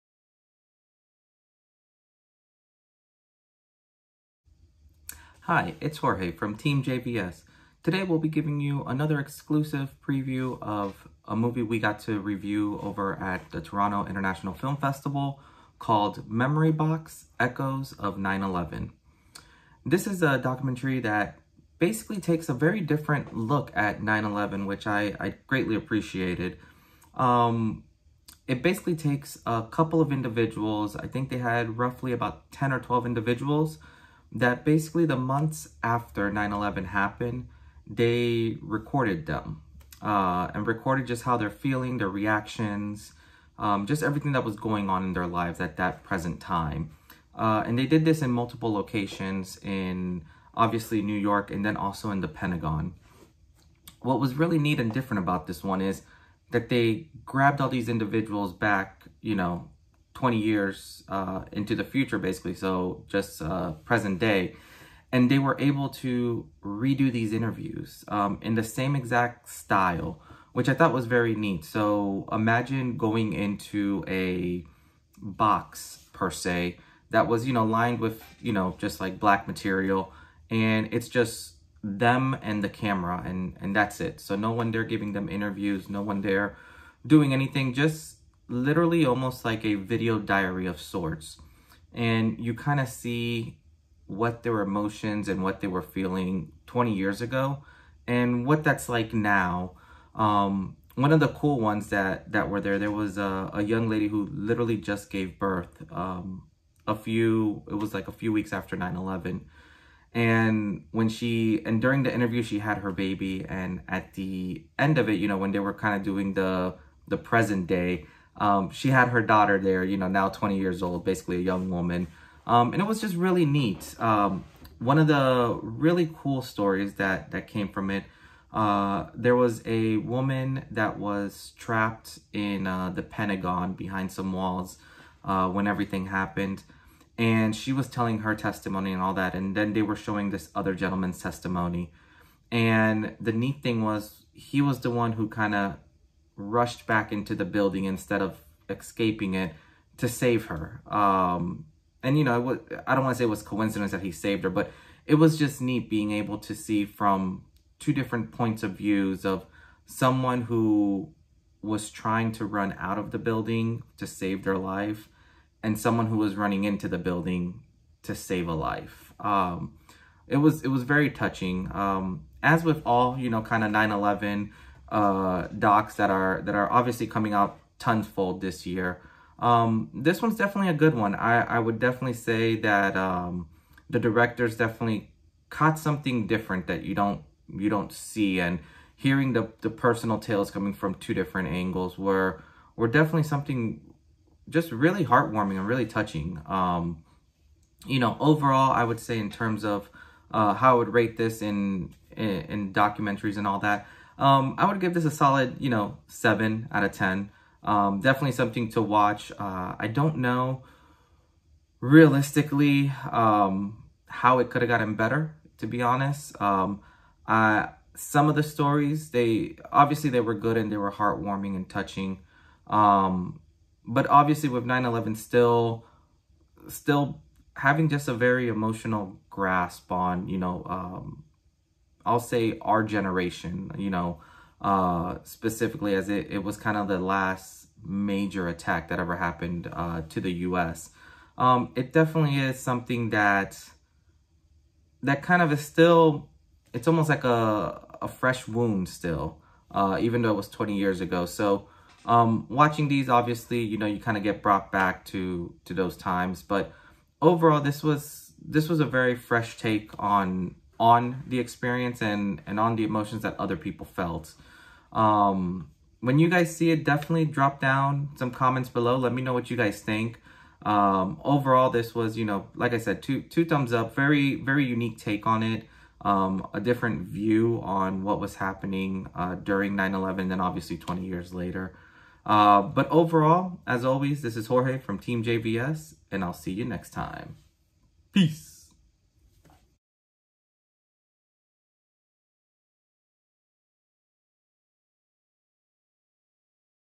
Memory Box Echoes of 911 Documentary Review TIFF 2021
Documentary collects accounts of 9/11, recorded in the months after the attacks, and present-day testimonials from the same eyewitnesses.

Directors
David Belton
Bjorn Johnson